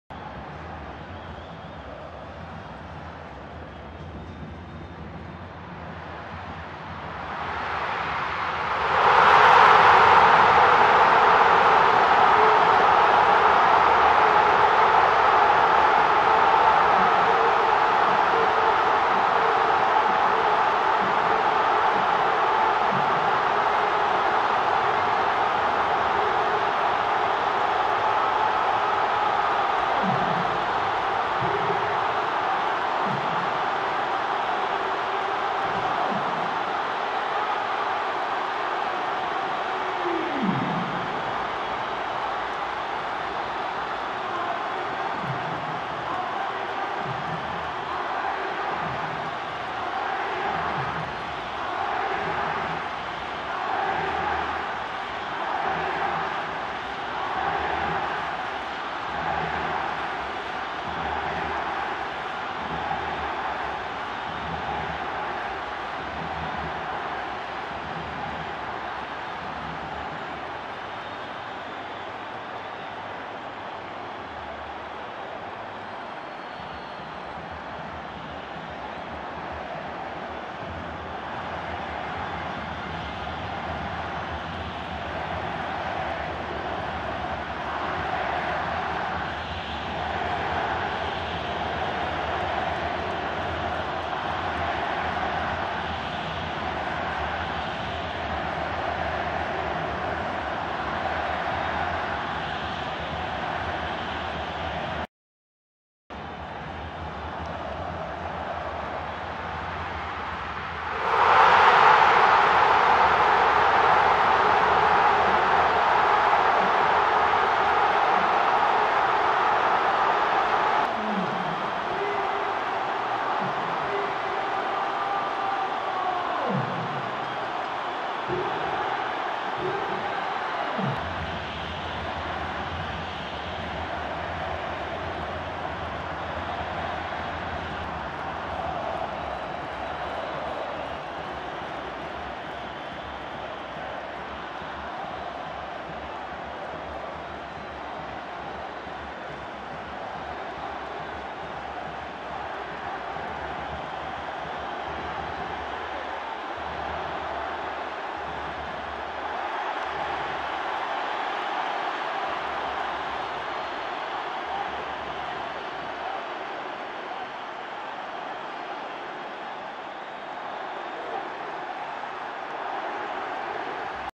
América vs. Santa Fe (2-0) | Liga Aguila 2019-II | Cuadrangulares Fecha 6
Los 'diablos rojos' derrotaron 2-0 a los 'cardenales' con goles de Matías Pisano y Michael Rangel.